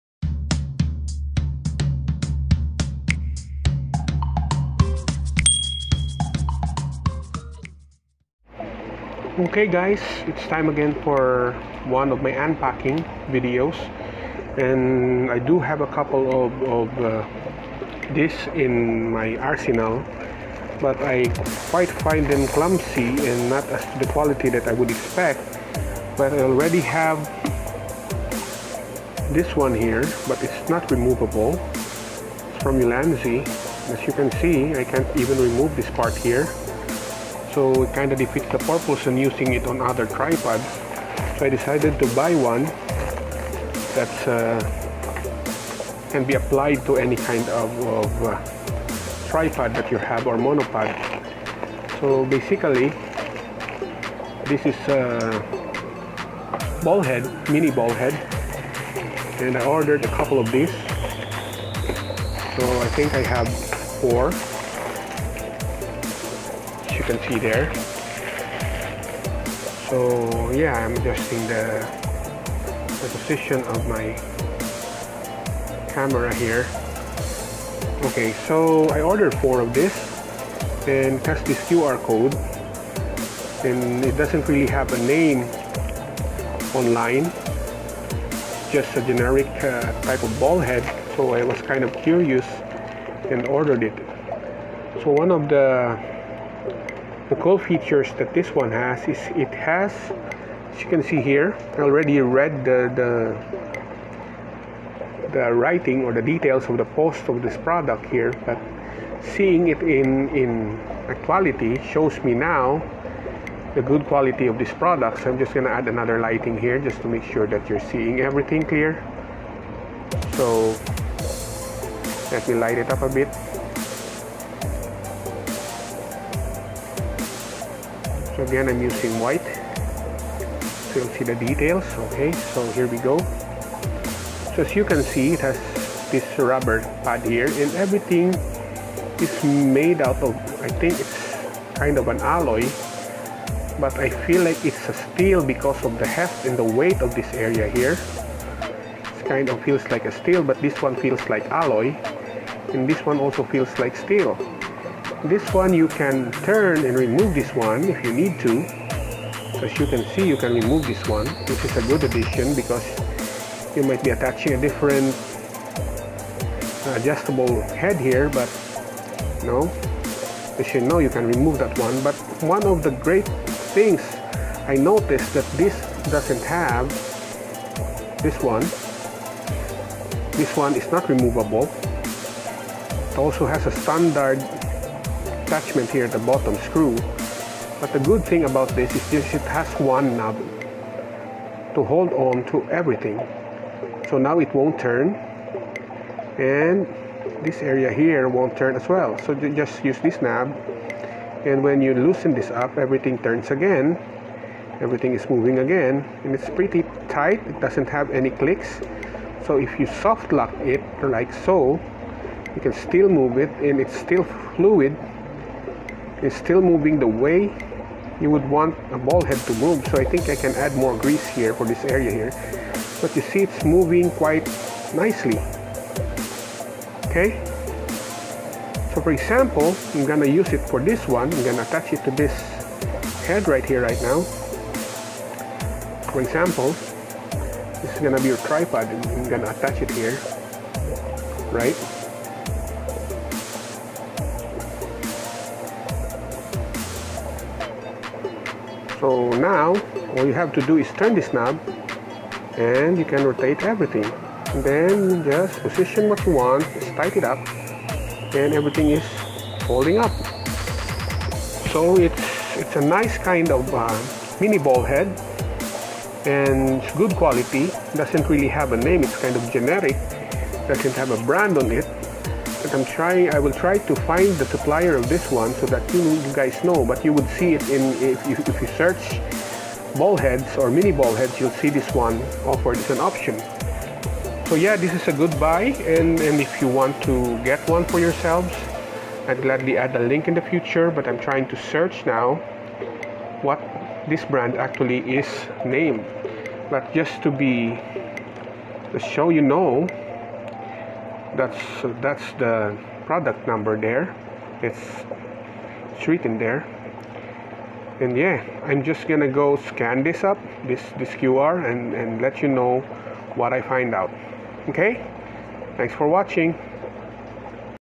Best Affordable Mini-Ballhead So Far
This ball heads can withstand heavy loads, lock solidly, and move smoothly.

Single Dial-Knob control.

Portable and Pocketable

This product can install 90% micro projectors on the market.

Anti-skid rubber pad gives your device a better contact protection.